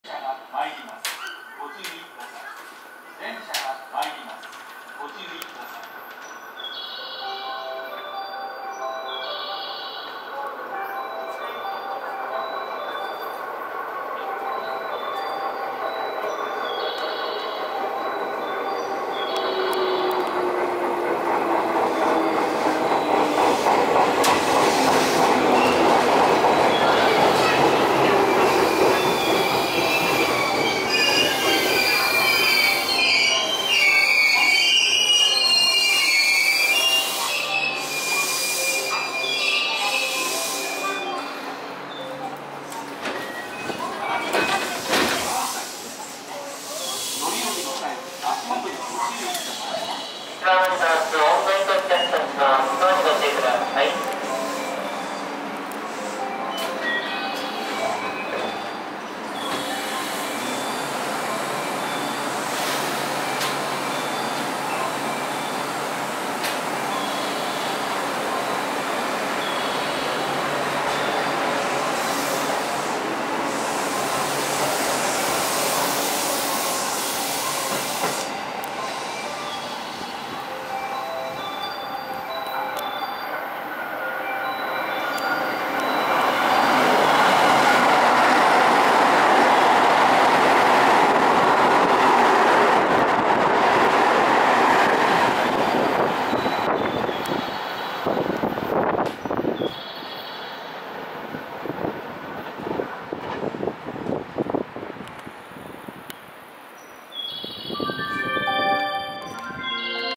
JR西日本 221系 K06編成 湖西線 B普通 近江舞子行き 223系2000番台 V54編成+V40編成 A新快速 大阪行き 唐崎駅 20221112
JR西日本

221系 K06編成
湖西線 B普通 近江舞子行き

223系2000番台 V54編成+V40編成
A新快速 大阪行き

唐崎駅